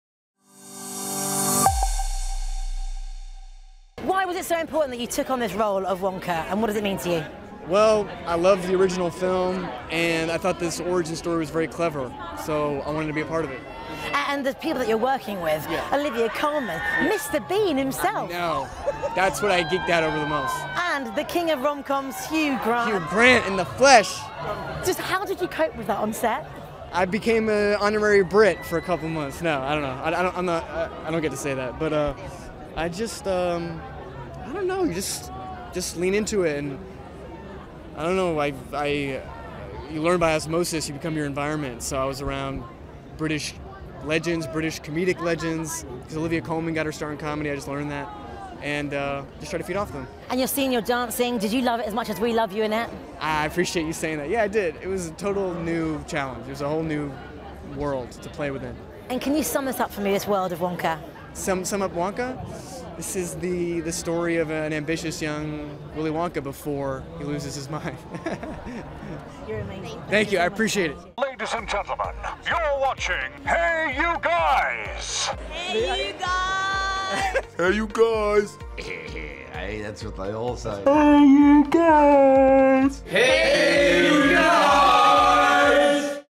Timothée Chalamet - Wonka Red Carpet Interview - London Premiere
Timothée Chalamet is interviewed on the red carpet Premiere for Wonka. Directed by ‘Paddington’s’ Paul King, the movie stars Timothée Chalamet, Calah Lane, Keegan-Michael Key, Paterson Joseph, Matt Lucas, Mathew Baynton, Sally Hawkins, Rowan Atkinson, Jim Carter, Natasha Rothwell, Rich Fulcher, Rakhee Thakrar, Tom Davis, Kobna Holdbrook-Smith, with Olivia Colman.

Plot: Based on the extraordinary character at the centre of ‘Charlie and the Chocolate Factory,’ Roald Dahl’s most iconic children’s book and one of the best-selling children’s books of all time, “Wonka” tells the wondrous story of how the world’s greatest inventor, magician and chocolate-maker became the beloved Willy Wonka we know today.

The movie hits cinemas on December 8th.